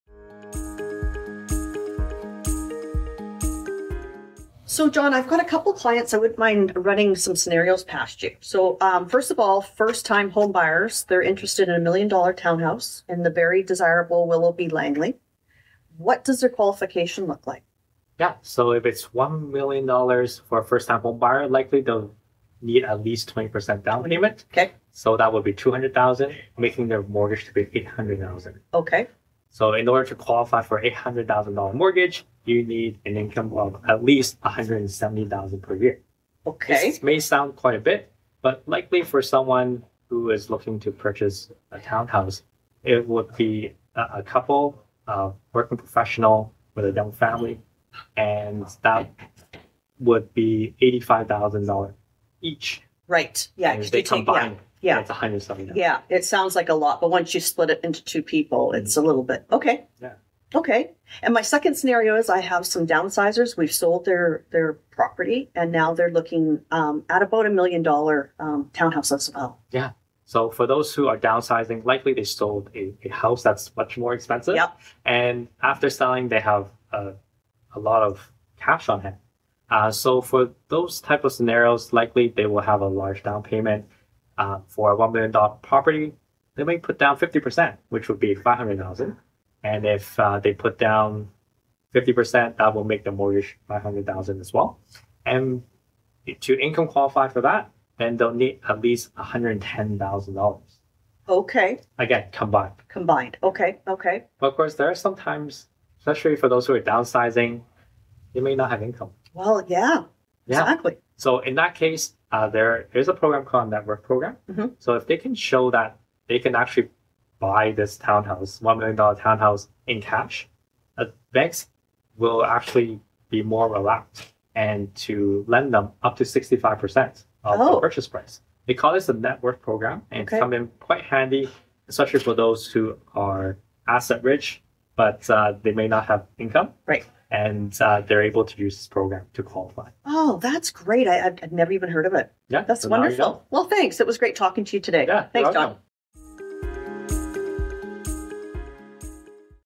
How Much Income Do I Need To Qualify For A $1,000,000 Home Purchase?
Thinking of making a big purchase soon? 👀

Join Tana McNicol from Royal LePage, as her and John dissect the income requirements needed to be eligible for a $1 million purchase in today's housing market 🏡